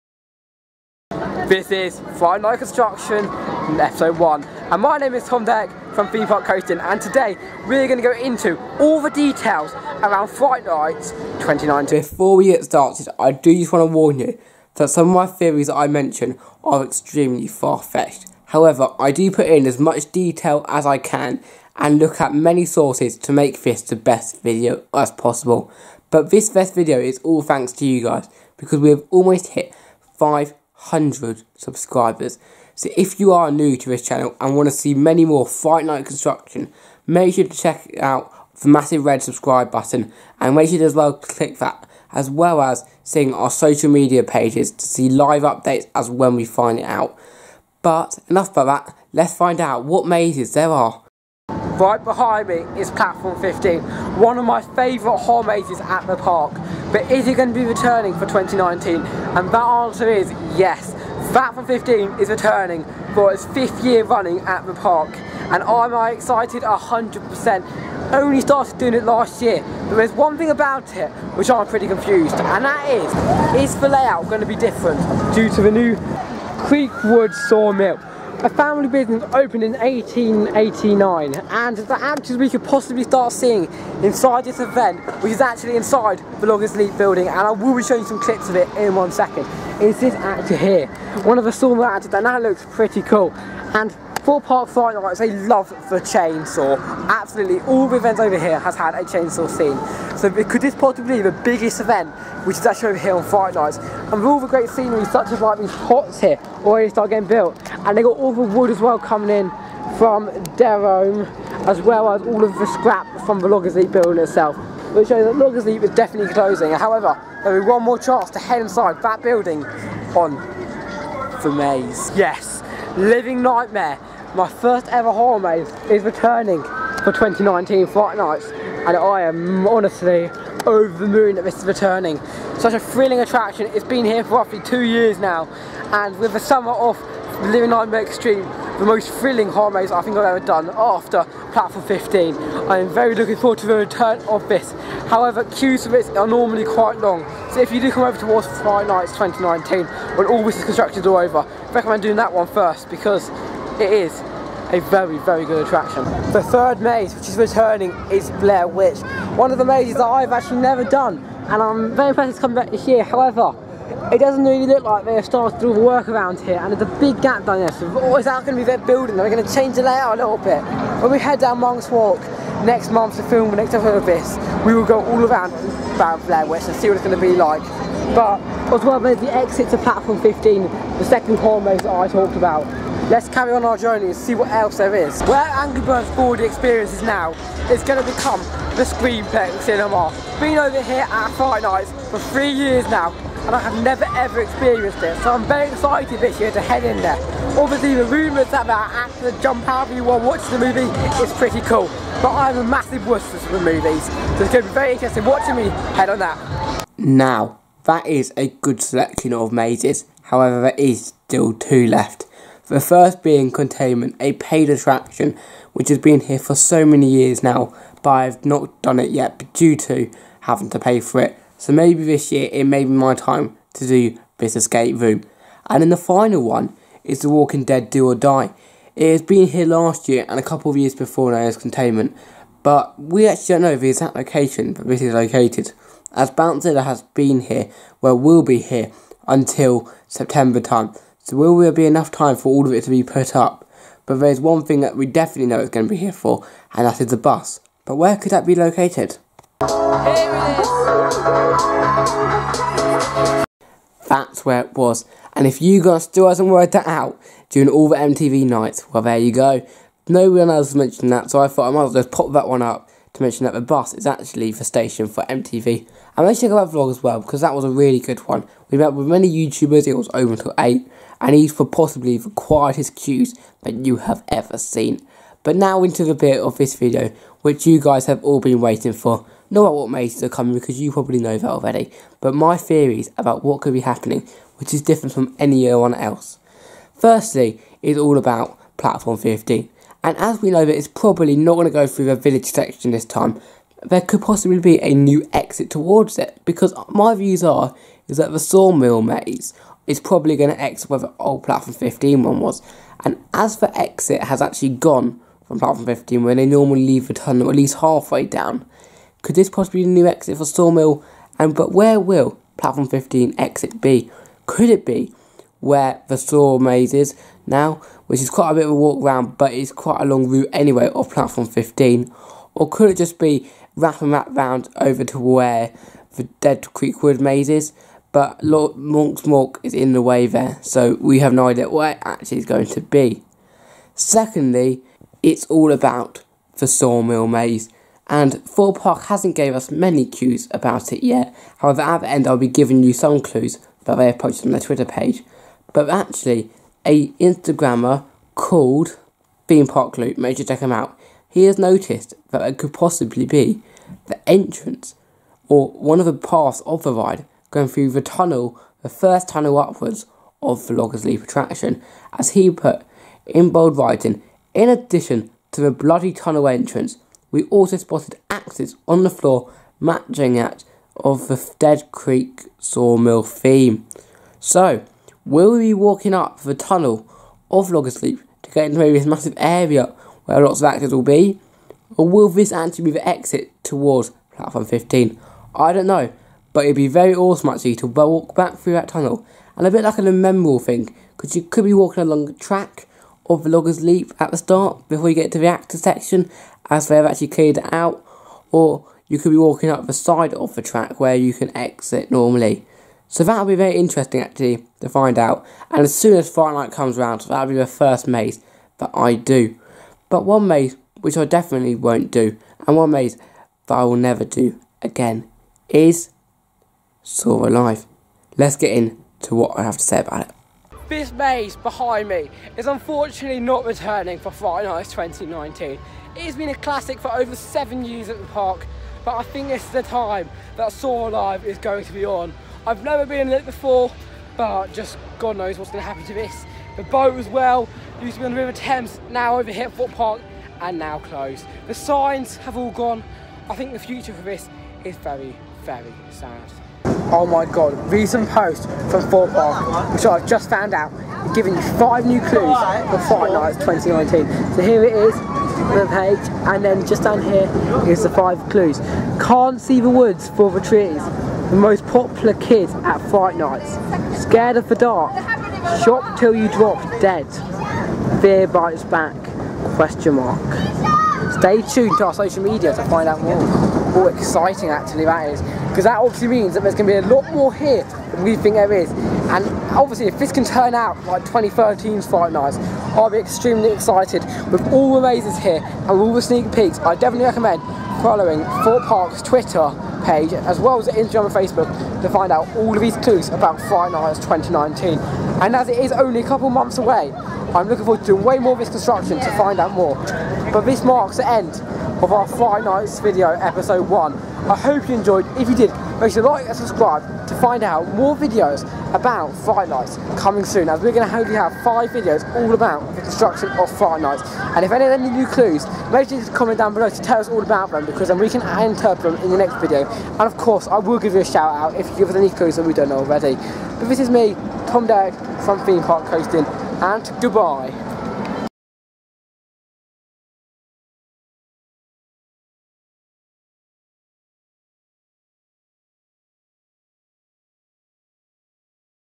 Fright night construction August 2019 | Thorpe park
Join us at thorpe park resort as we head into the construction of the biggest event of the year 'fright night' and take a look at many clews aswell as giving lots of speculation around it.
LOCATION
Staines Rd, Chertsey KT16 8PN
VIDEO STARS
Tom Derrick